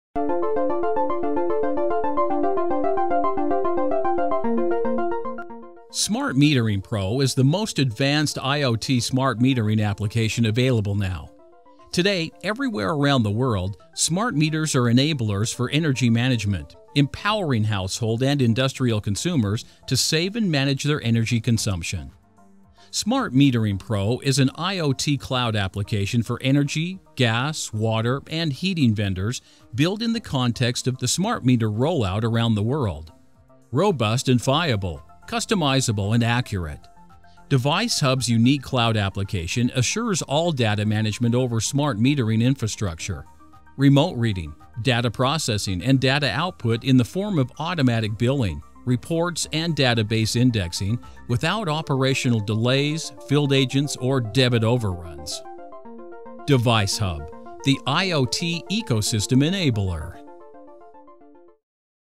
What is DeviceHub Smart Metering Pro
Get to know DeviceHub`s Smart Metering Pro, the most advanced cloud solution for smart meter data management.